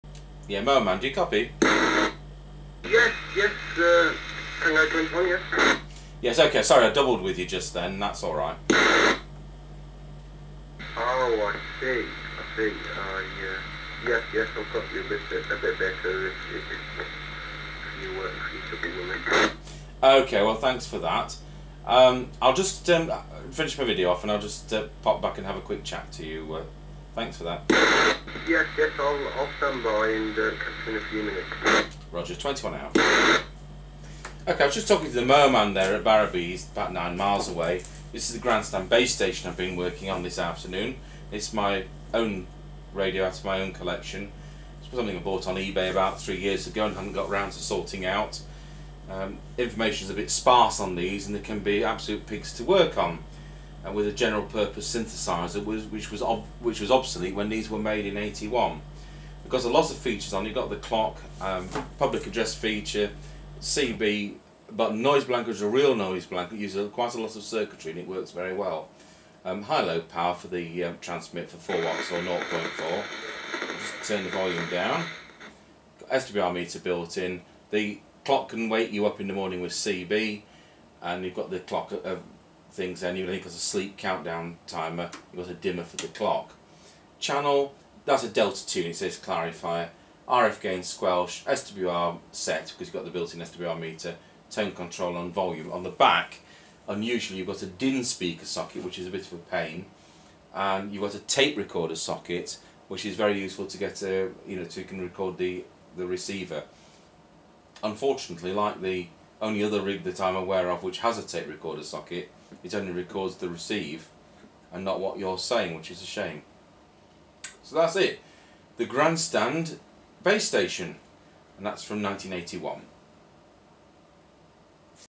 Grandstand Base Station, UK (CB 27/81) CB radio (Base Station) - On The Air Test
Richard Shireby, G0OJF, from TSG Communications, Grantham, Lincs (UK), discusses & demonstrates alignment of the Grandstand base station, manufactured in Japan by Suruga in 1981. Capable of good results, these unusual sets can be a pig to set up correctly, not helped by lack of service information. I do have a circuit, luckily...but that's all. The NEC made UPD2812C synthesiser IC that these sets use was already obsolete when these sets were made. I assume that Suruga had bucket loads of them to use up! The tape recorder output socket on the back is a great feature. Reliability can sometimes be a let-down on these rigs because of the double sided PCB being linked through with ordinary tin plated wire, this goes dry joint...usually under the wax-filled IF/synth screening can. You can spend some happy time with a hair dryer trying to get enough of the wax out to fault find!

The following is a link to a conversation I had with The Electric Man, thereby demonstrating the transmitted audio of this radio.

36Bxm3yfnPk